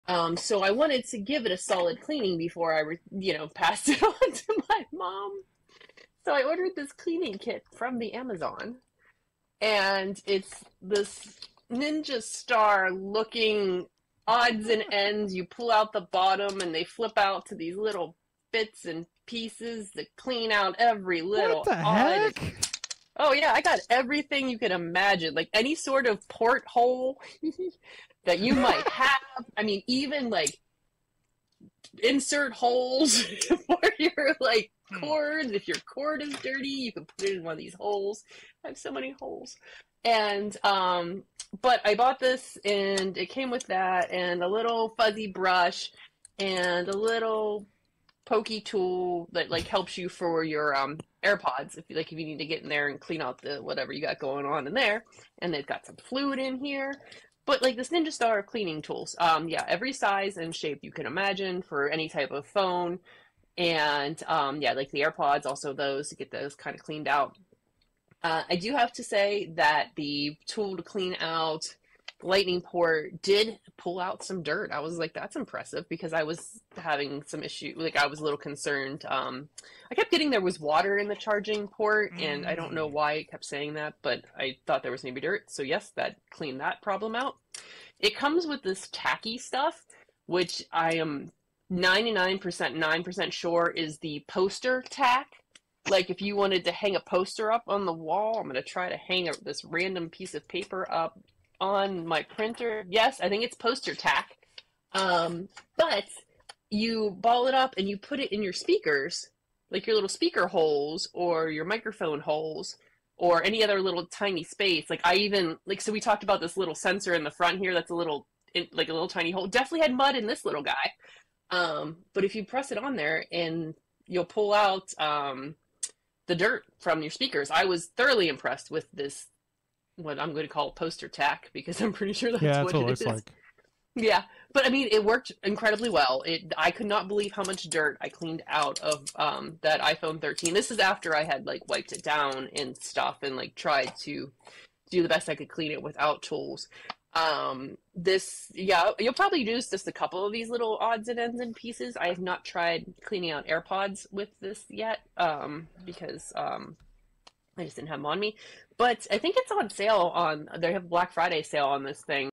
I Didn't Realize How Dirty My Phone Was Until I Used This Genius Cleaning Kit
What is the ninja star cleaning kit? A phone cleaning kit with multiple attachments that fold out in the shape of a ninja star to clean phone ports and holes.

What does it include? Small brushes, poke tools, cleaning fluid, and tacky material to clean out phone speakers, microphones, charging ports, headphone ports, and other small spaces.

What phone issues did it resolve? It pulled dirt and debris out of the lightning port that was causing charging problems on an iPhone.

What surprised the user about it? The amount of dirt that was pulled out of the phone, even after attempting to clean it manually first.

Why is the user impressed? It thoroughly cleaned out dirt and debris from small phone spaces they didn't expect anything to really reach, resolving a charging issue.